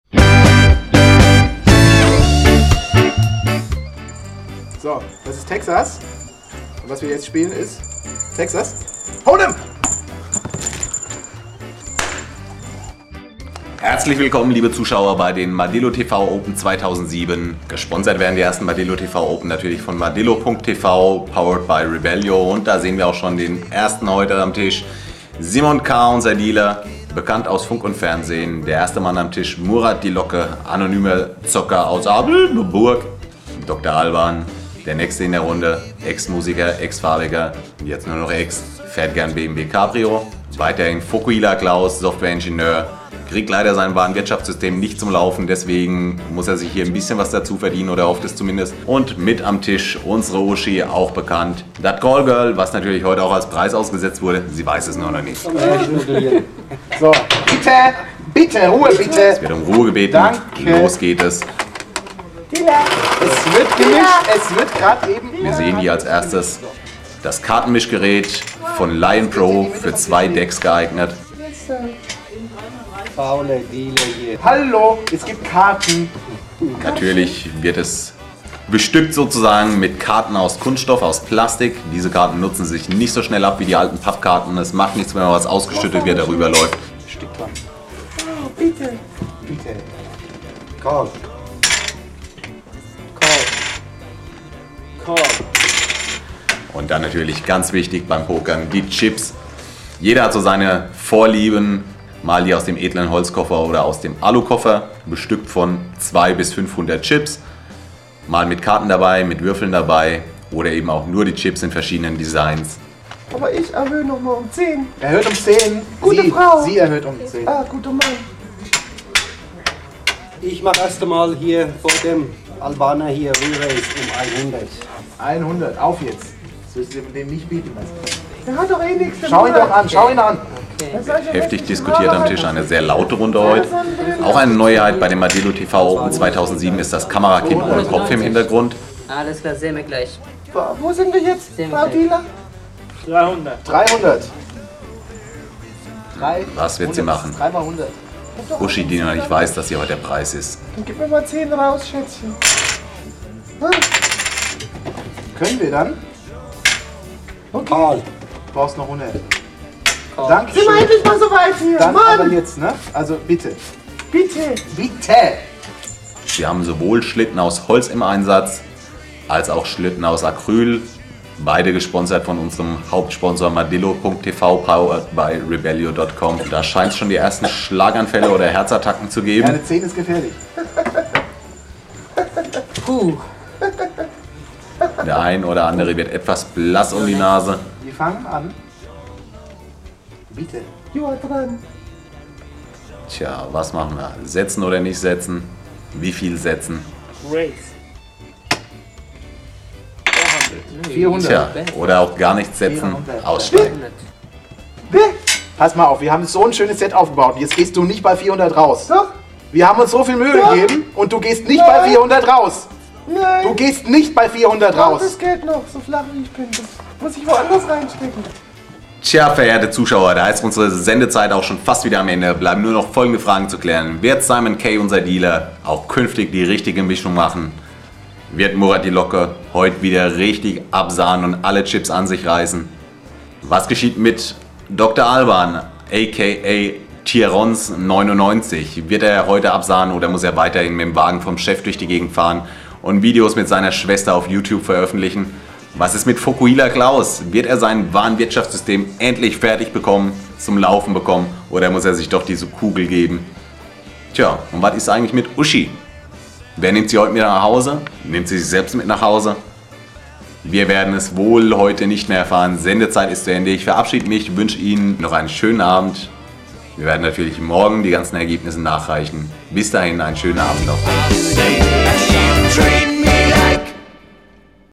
madillo.tv Folge19: Texas Hold`em
Die madillo.tv-Open 2007, das erste Poker-Turnier der verrückten Jungs aus Aschebersch...